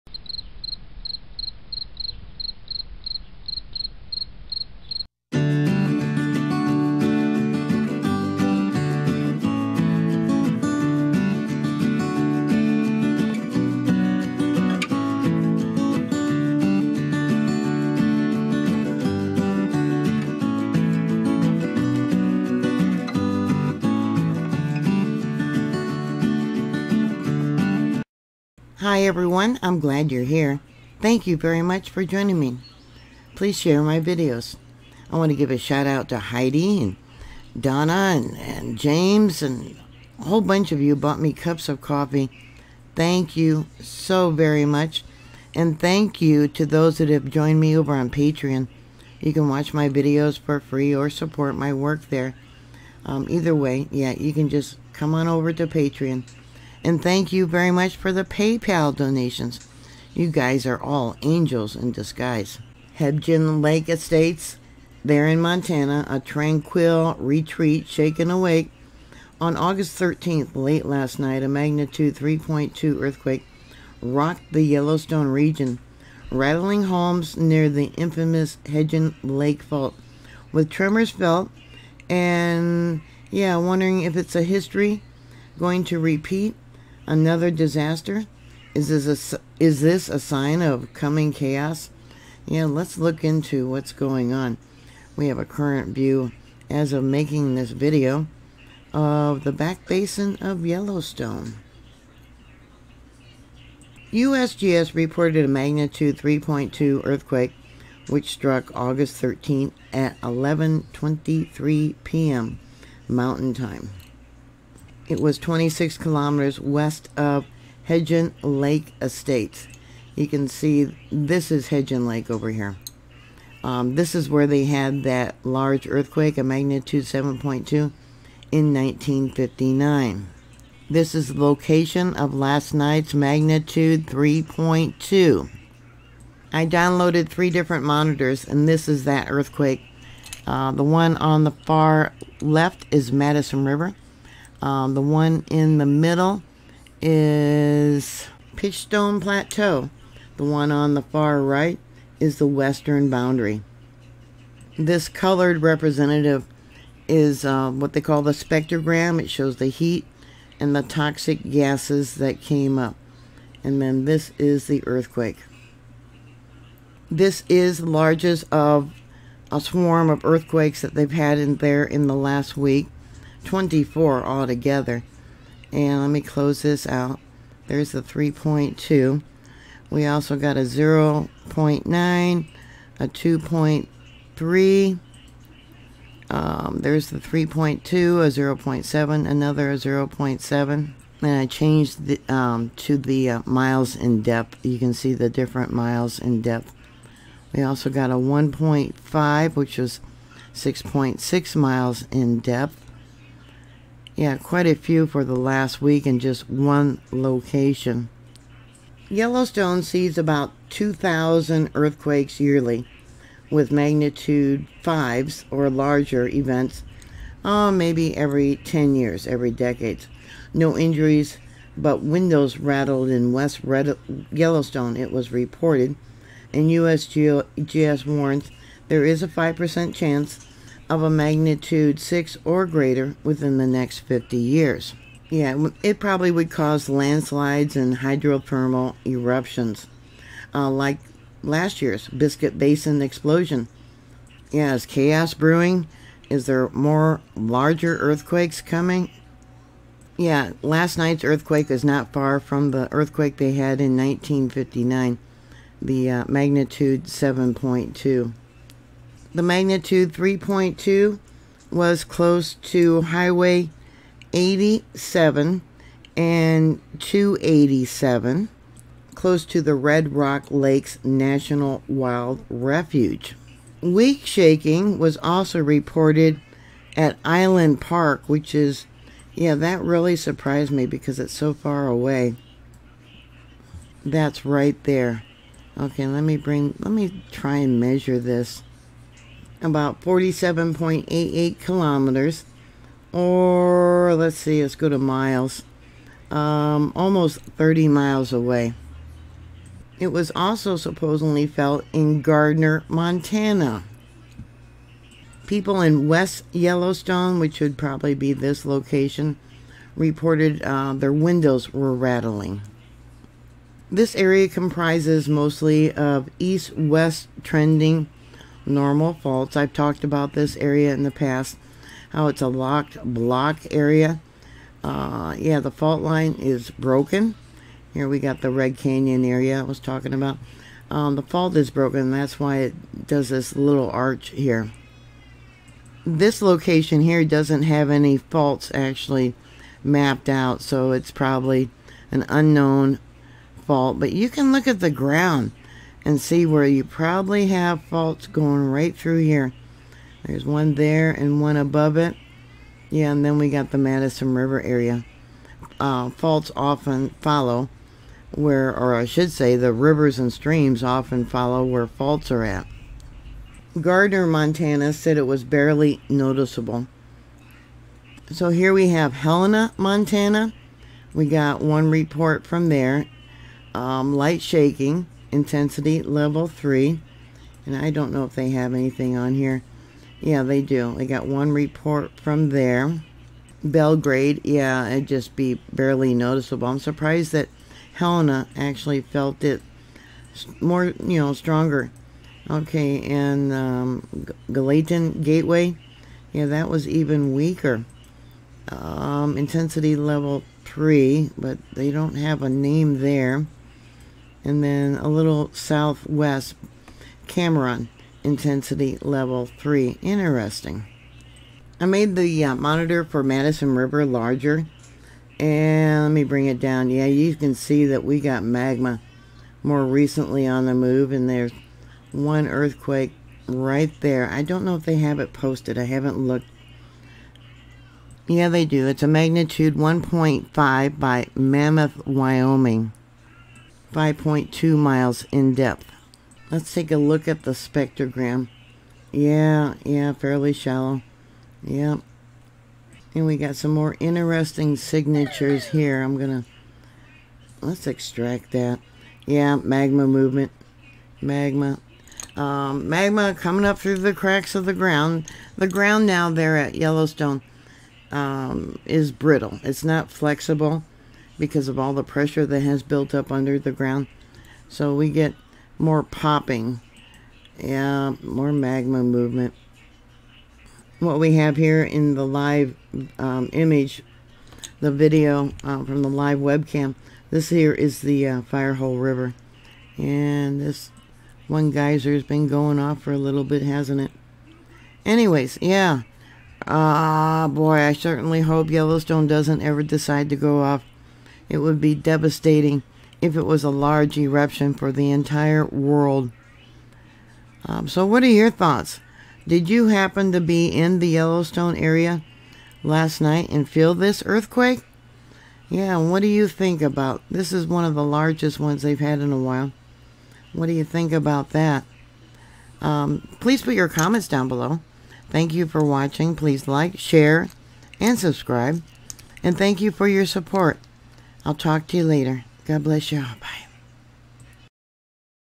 Hebgen Lake’s M3.2: Yellowstone Faults Rumble
Hebgen Lake Estates, Montana—a tranquil retreat shaken awake. On August 13, 2025, a magnitude 3.2 earthquake rocked the Yellowstone region, rattling homes near the infamous Hebgen Lake Fault. With tremors felt and history warning of disaster, is this a sign of chaos? Let’s dive into the seismic storm.

Inquiries
Mary Greeley

M 3.2 - 26 km W of Hebgen Lake Estates, Montana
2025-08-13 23:23:10 (UTC)44.812°N 111.519°W12.5 km depth